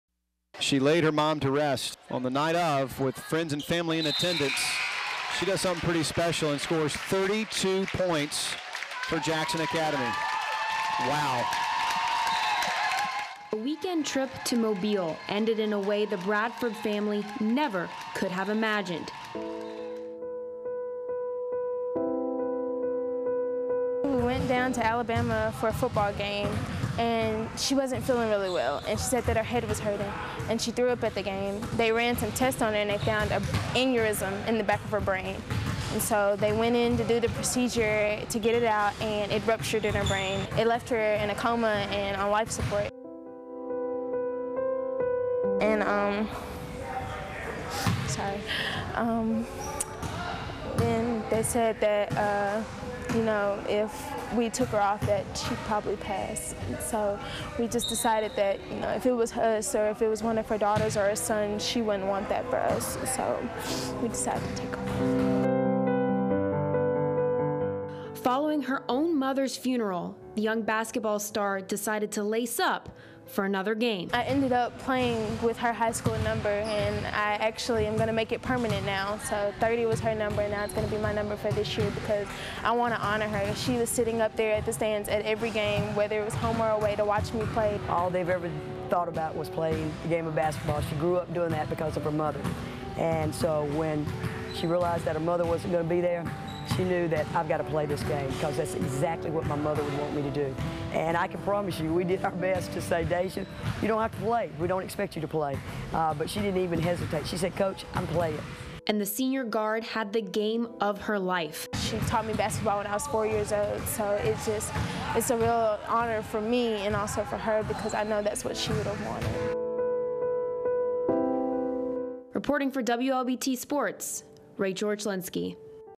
From a parent's funeral to the court
Imagine attending a funeral for a parent in the morning, then dropping a career-high 32 points that night...
De'ja Bradford did that. Her story is beyond inspirational.